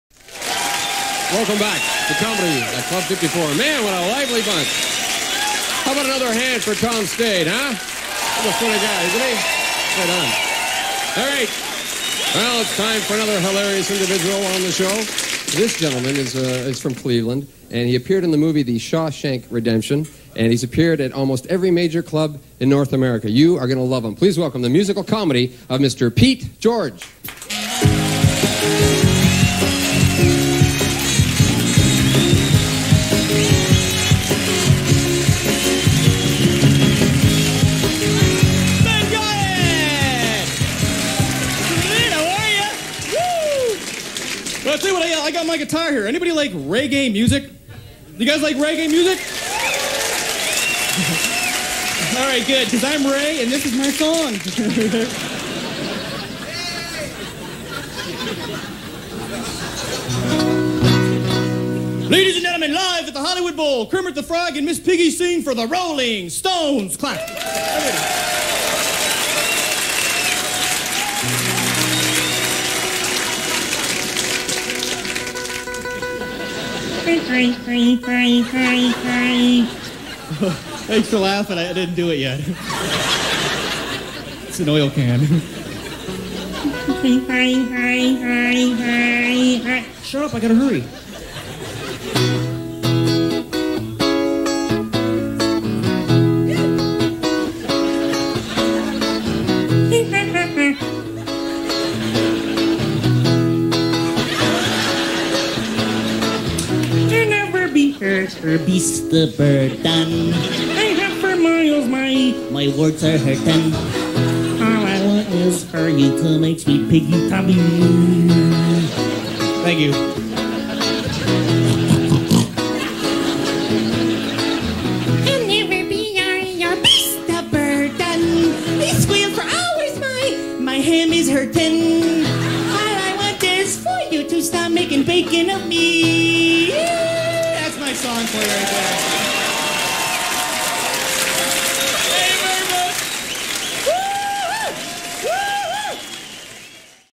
PETE GEORGE (comedian/Musician) Comedy Club 54 ( teaser clip )
Pete George is a well know American Comedian, Actor, Writer and a very talented musician.. Here is a small clip pf PETE performing at Comedy Club 54 in Ontario back in the mid 90's